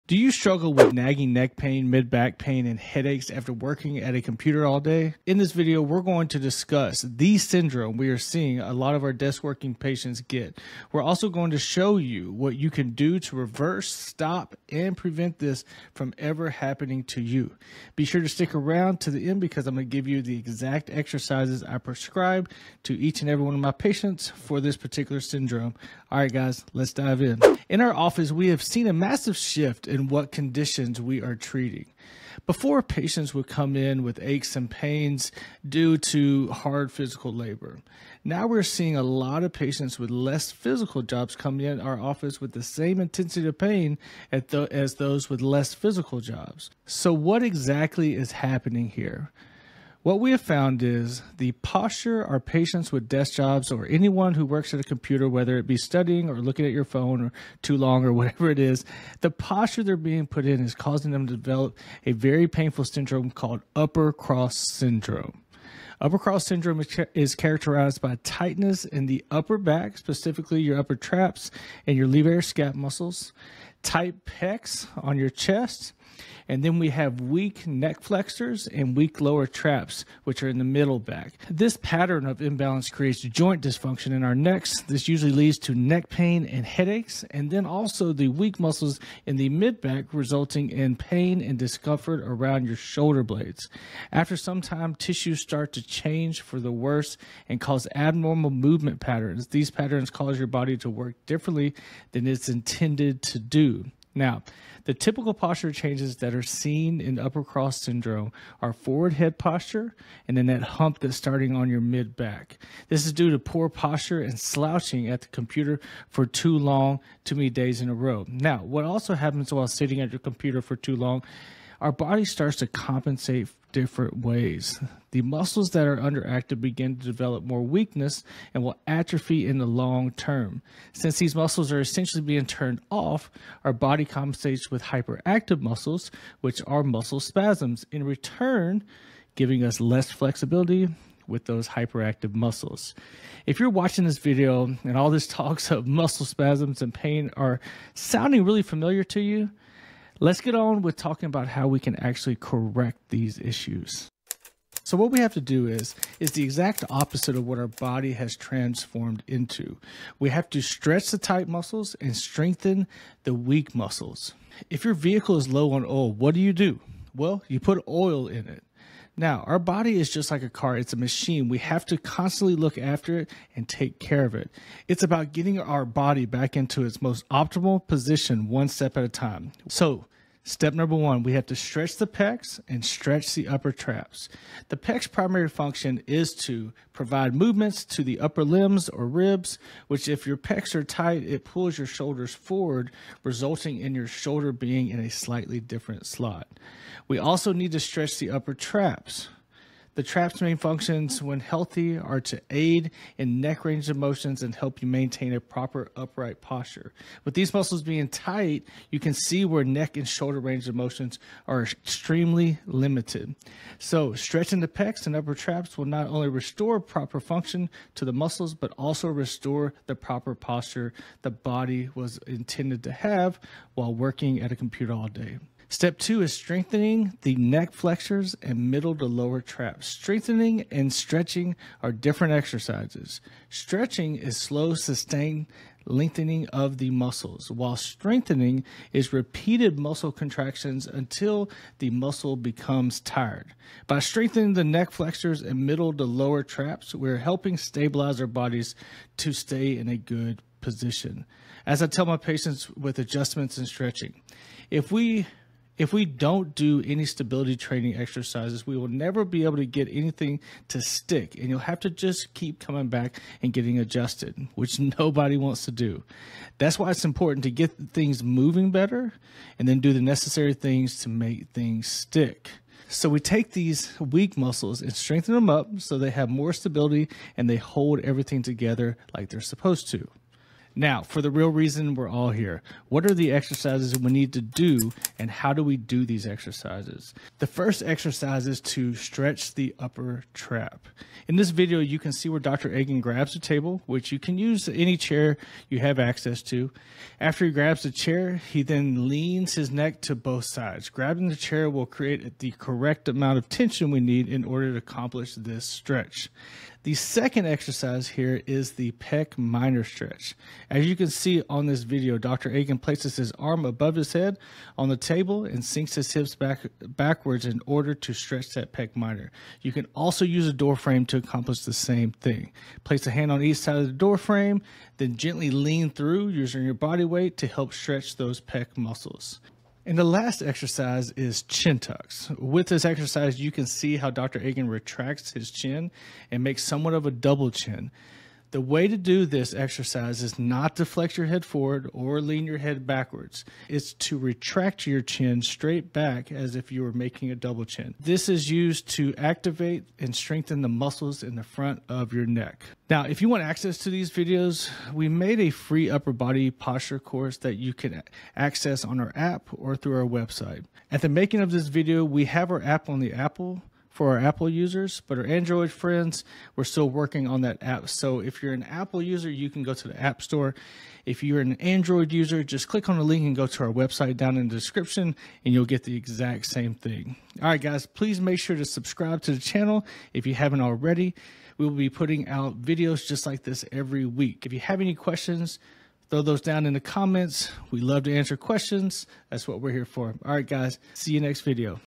The Painful "Syndrome" affecting Desk Workers
Make sure to either download the our StraightUp Posture App or access the FREE Upper Body Posture Course through our website!

In this video, we dive into Upper Cross Syndrome (UCS), a common condition affecting many desk workers today. If you spend hours hunched over a computer, you might be at risk of developing UCS, a postural imbalance where certain muscles become tight while others weaken. This imbalance can lead to neck, shoulder, and upper back pain, making daily tasks uncomfortable and reducing your overall quality of life.

We’ll explore:

✔ What Upper Cross Syndrome is and why it happens.
✔ The common symptoms and signs to look out for.
✔ How prolonged sitting and poor posture contribute to UCS.
✔ The impact of UCS on your daily life, especially for those with desk jobs.
✔ Simple exercises and stretches to help alleviate pain and correct muscle imbalances.

Whether you're already experiencing discomfort or looking to prevent future issues, this video provides practical tips to help you maintain a healthy posture and avoid the pain associated with Upper Cross Syndrome.

00:00 Introduction
00:28 The "Sydrome" Explained.
02:33 How to Correct this Syndrome
04:43 The Exercises you should be doing